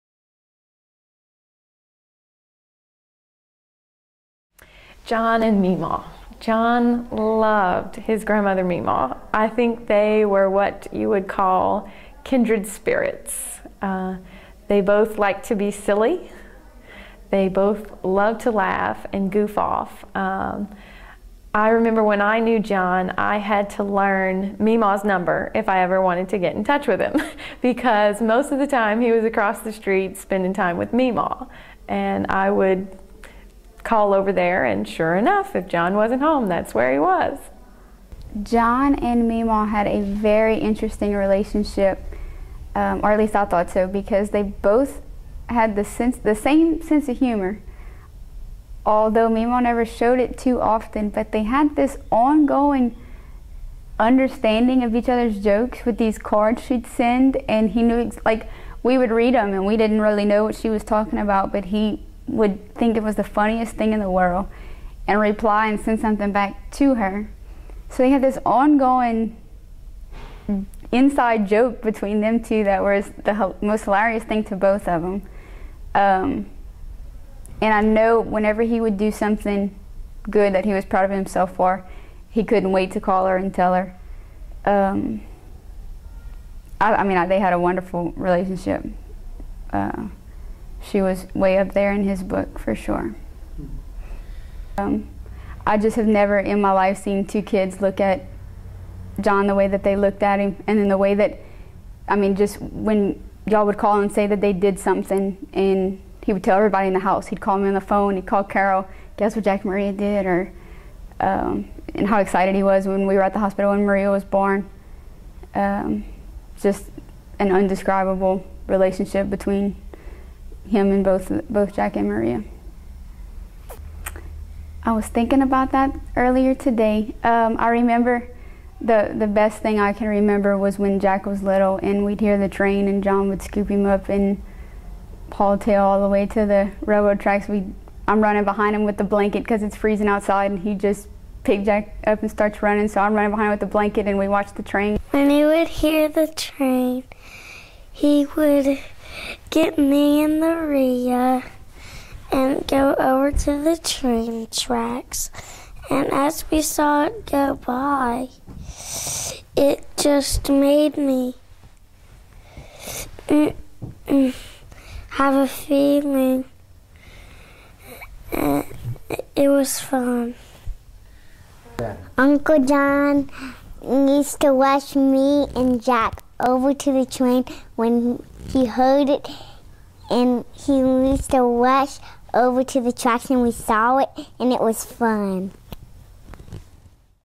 John Melvin Movie - Chapter 4 - Memaw, Jack, & Maria
John and Mema were kindred spirits and he had an indescribable relationship with Jack and Maria.